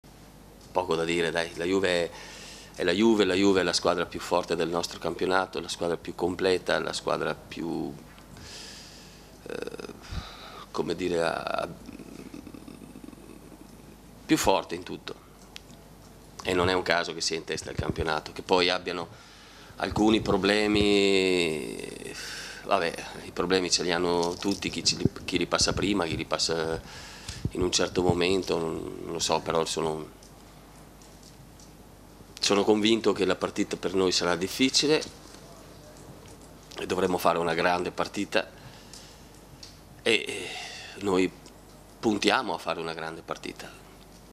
Juve-Udinese, Guidolin: "Faremo una grande partita"
Nella conferenza stampa prima della sfida allo Juventus Stadium, Guidolin ricorda a tutti che la squadra di Conte resta la più forte del campionato nonostante il momento difficile, ma l'Udinese punta a fare una grande partita.